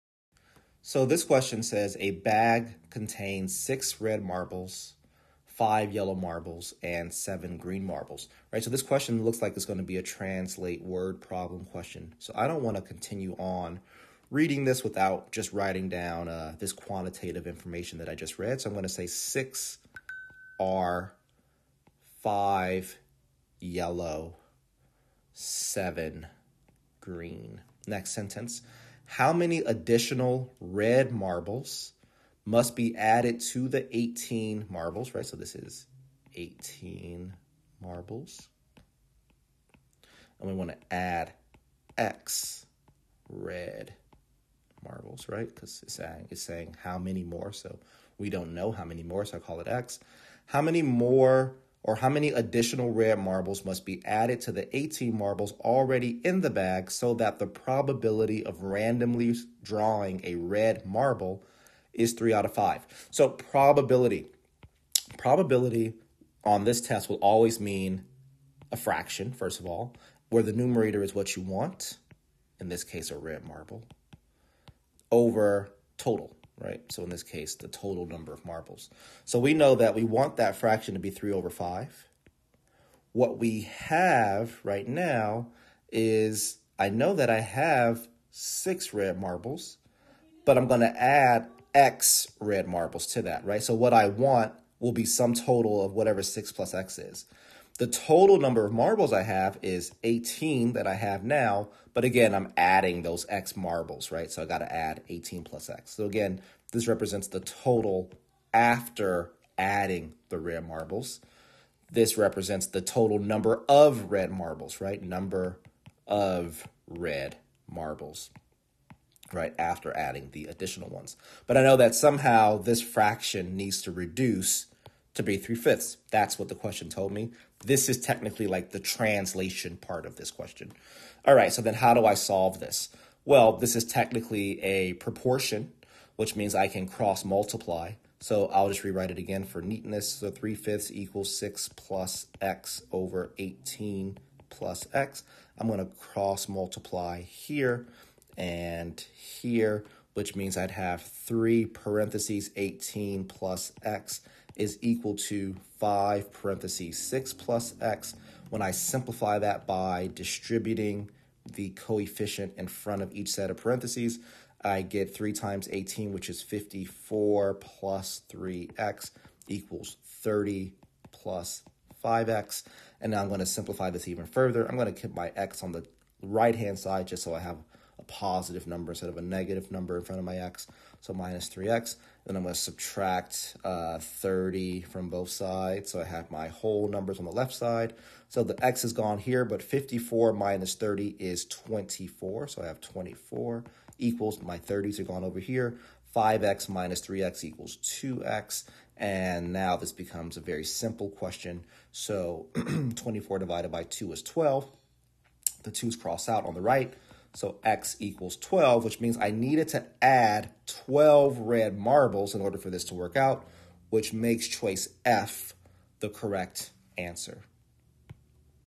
A bag contains 6 red marbles, 5 yellow marbles, and 7 green marbles. How many additional red..
2008 Official ACT Practice Exam question 24:

A bag contains 6 red marbles, 5 yellow marbles, and 7 green marbles.  How many additional red marbles must be added to the 18 marbles already in the bag so that the probability of randomly drawing a red marble is 3/5?